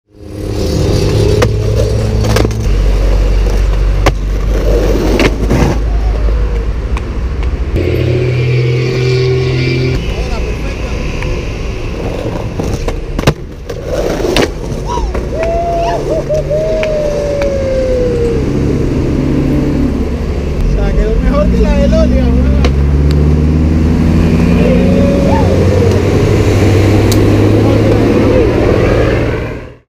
Miguel Castro N.B.D. - Skateboarding Panama
Miguel Castro coronando este N.B.D. en las areas de Arraijan.

skateboarding panama